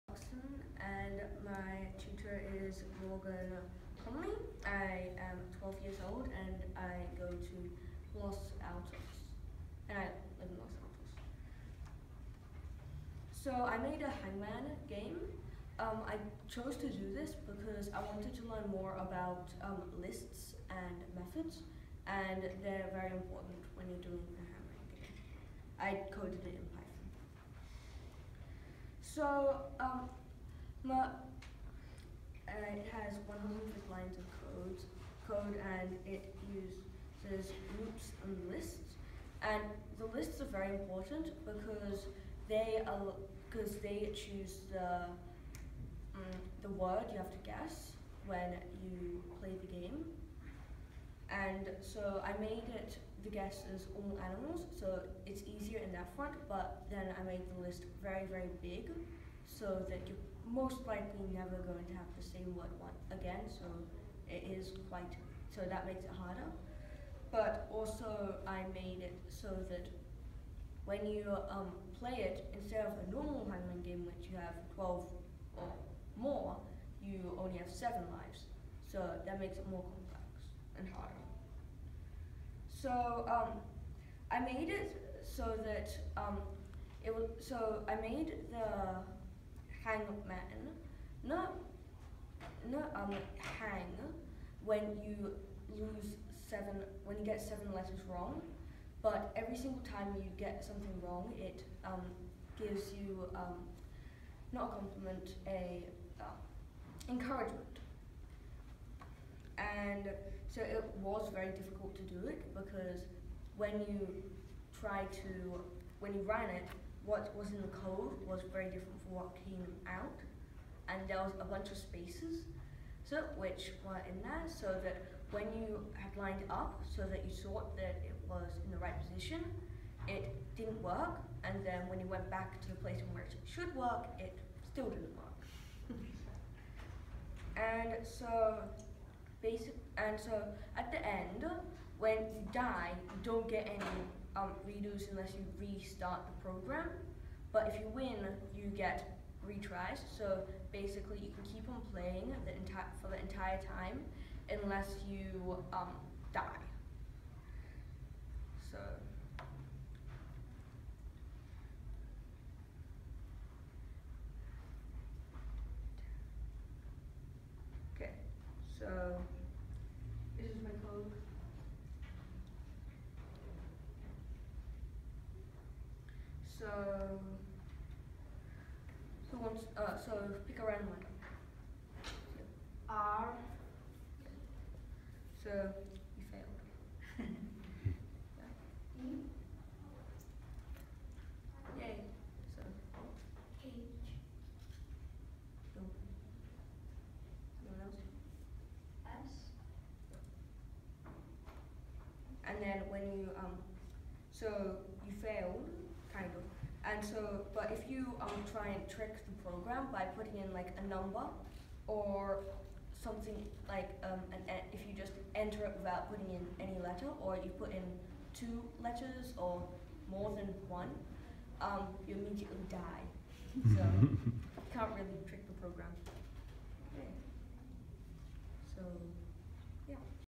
12 year-old programmed Hangman game in Python presentation
Part of the 2016 Breakout Mentors student showcase in Los Altos, California